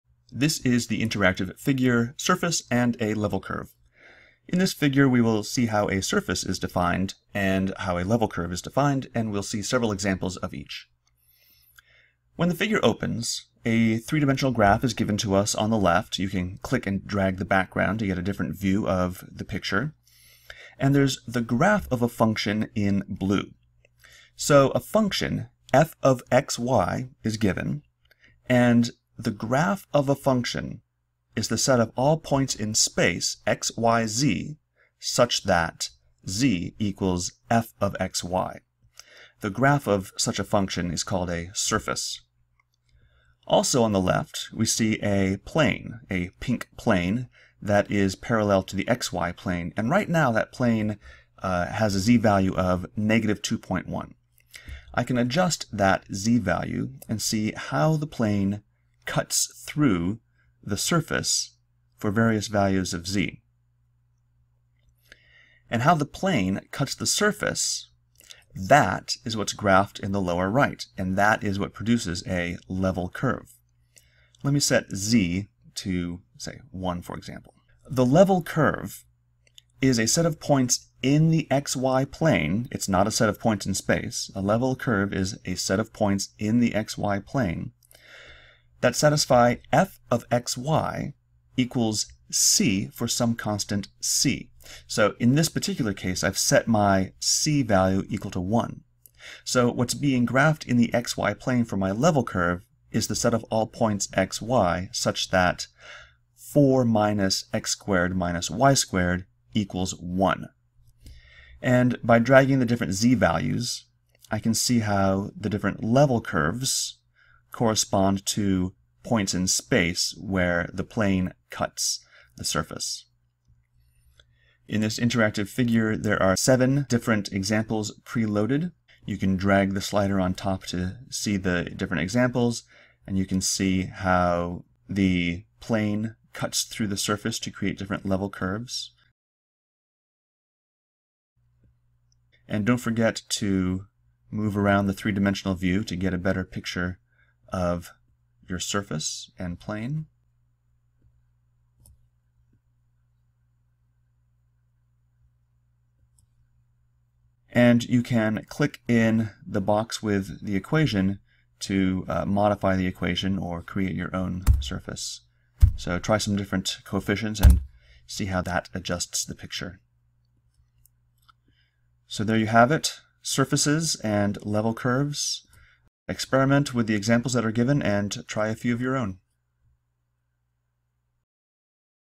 Visualizing Surface and Level Curves
Interactive Figures created for Thomas' Calculus 14e. Figures created by Marc Renault and Steve Phelps.